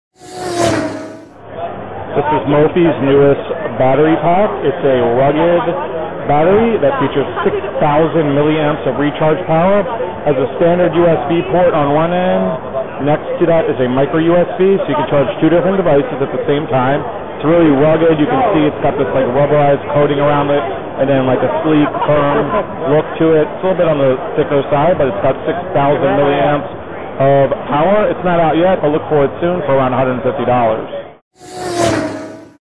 CES Digital Experience 2012: Mophie 6000 mAh Rugged External Battery
In this video I show the Mophie Rugged 6000 mAh External Battery.